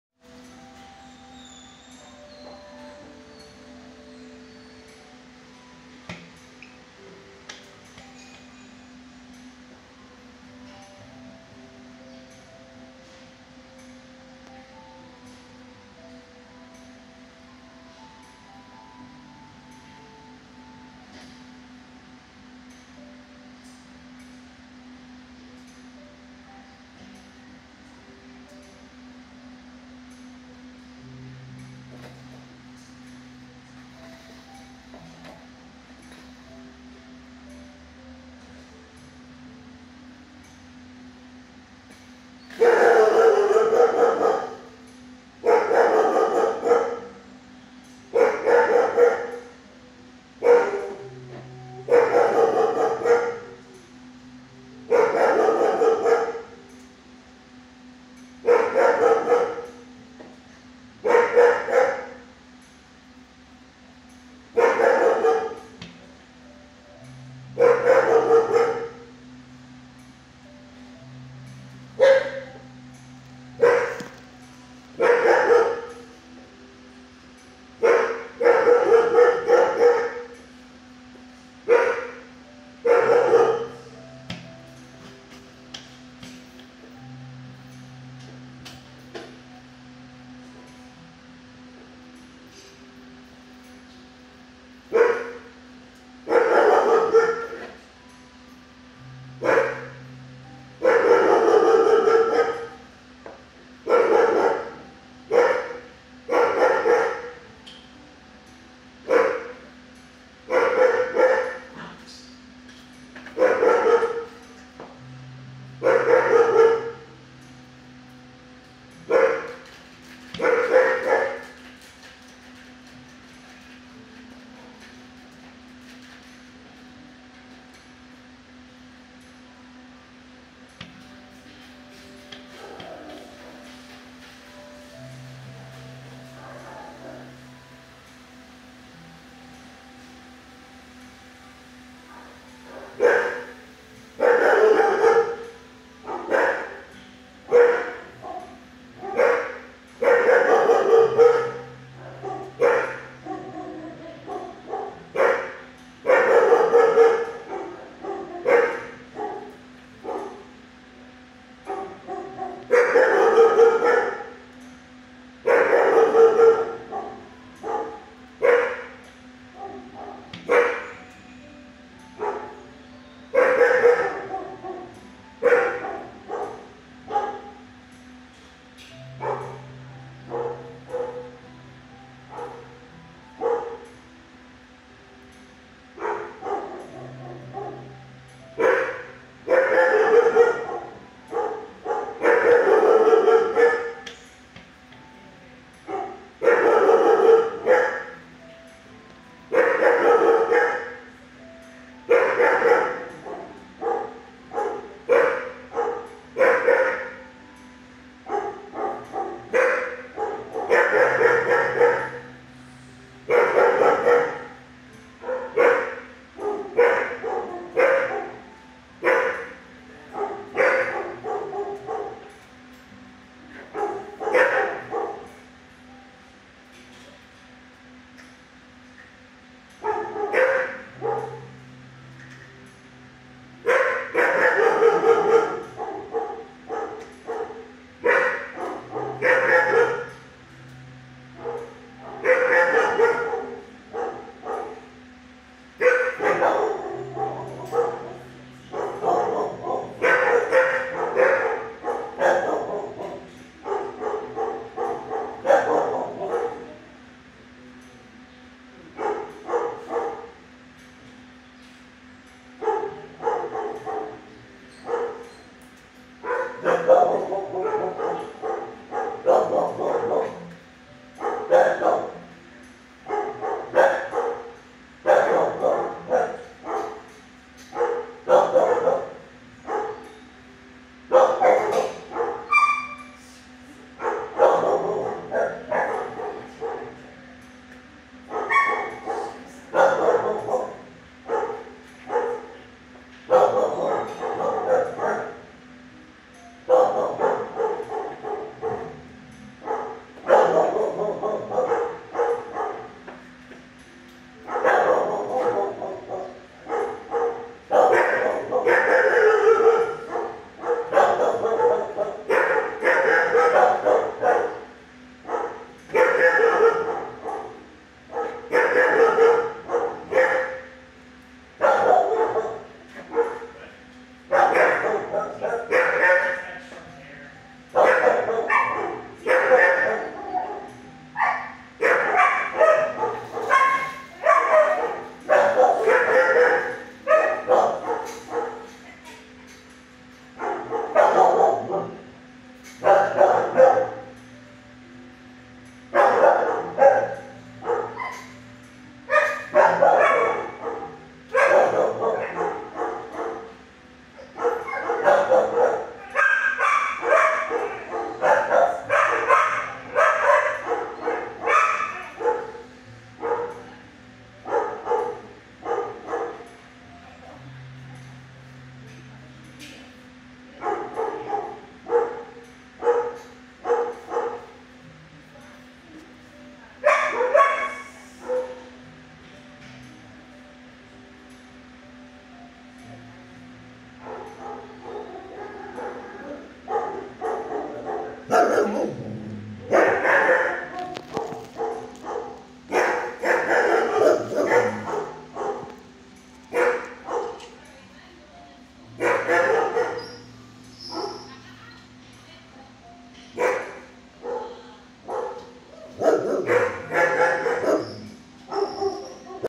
Angel 6y Pet Id 941860 kennel 33 HSSAZ shelter kibble offer shows my weakness fragile lady ponder652
She has always been uncomfortable with me and my offering her food only made me look weak in her eyes in the beginning. She needed reassurance that I was a leader and all would be okay, but my kibble offering did not signal strength. I'm looking only for her calmness & hopefully she will begin to feel more relaxed as we get together in the future(if she's not adopted before then). She is calming some as she laid down, but outside pressures began mounting so she begins barking and I decide I should leave before she gets really uncomfortable with me being around. My kibble offering to her will not help right now. Maybe some time way in the future giving her kibble might be an option when she much more comfy with me, but definitely not right now.